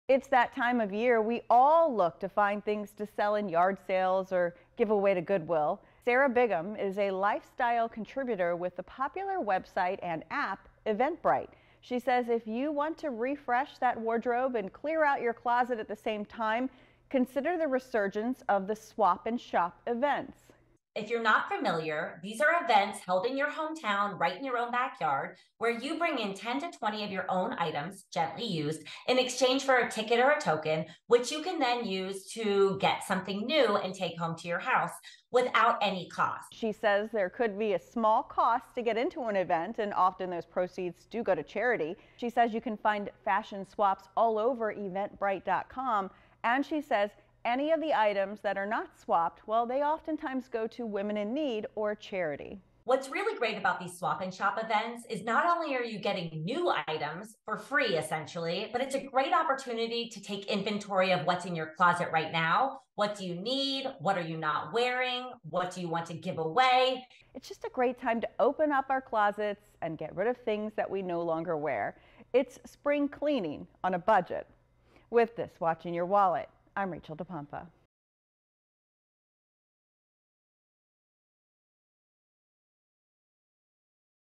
Fashion swaps are a low-cost way to refresh your wardrobe
Looking to refresh your wardrobe without breaking the bank? Consumer Investigator Rachel DePompa gets the lowdown on fashion swaps from Eventbrite.